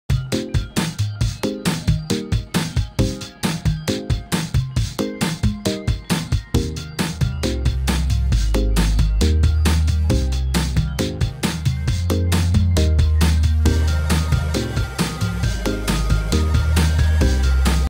Mini expressions test [+ cea’s new hairstyle]
The emoification of cea 💔/j

For real though, just when I thought cea couldn’t get more goth, she did. Now she’s a Victorian dressing mf and it’s in uncanny valley rn